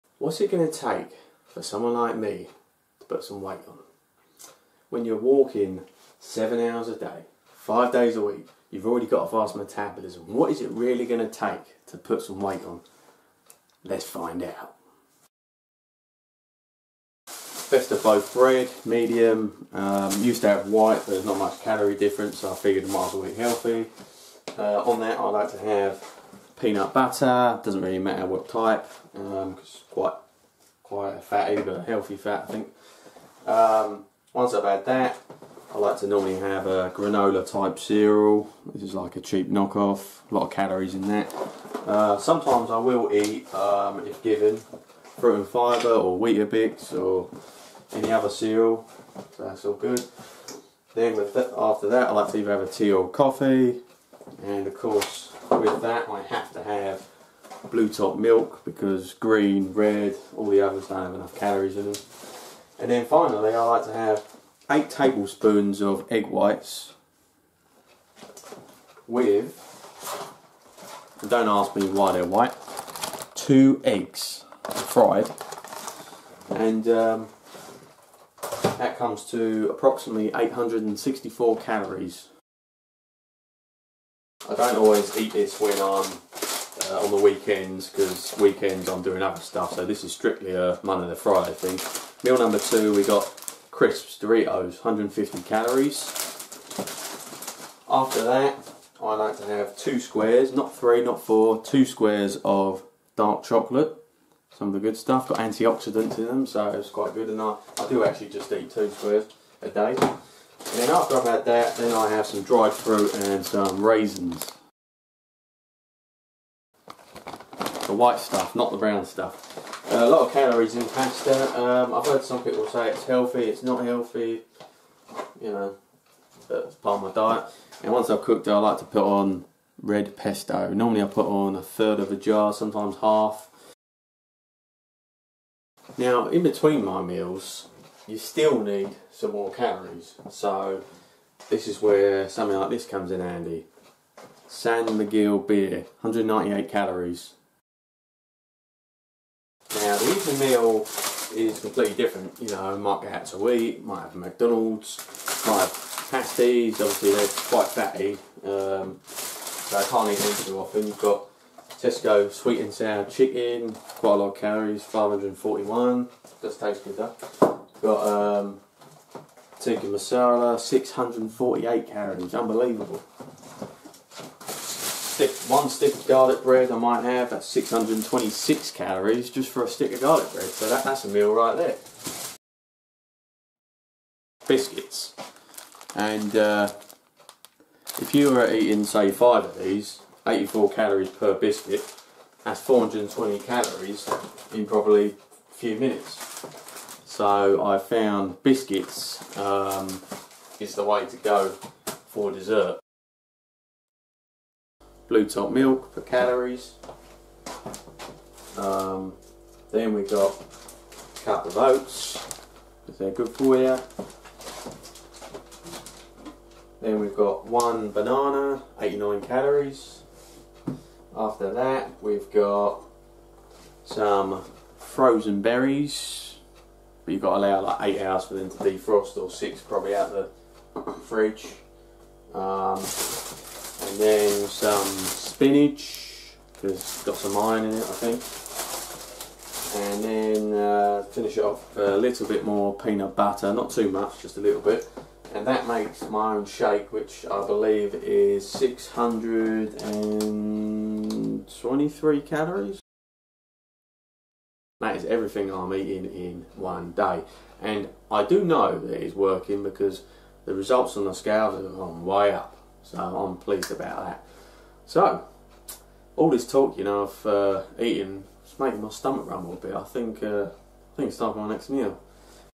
Gaining Weight with an Active Job
I thought it would be fun to show what I eat in a day to gain weight. I have a very active job that makes it hard to put weight on but I'm still trying.